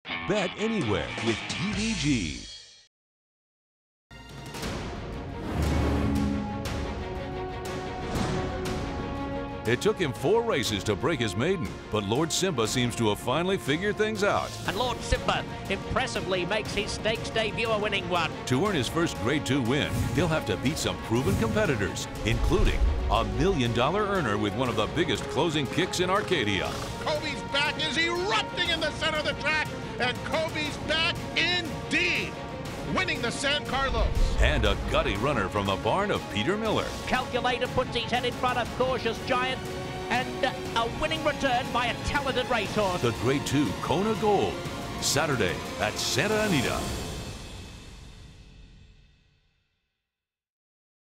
Lord Simba Leads Kona Gold Field
Lord Simba Heads a competitive field of sprinters in Saturday's Kona Gold Stakes at Santa Anita.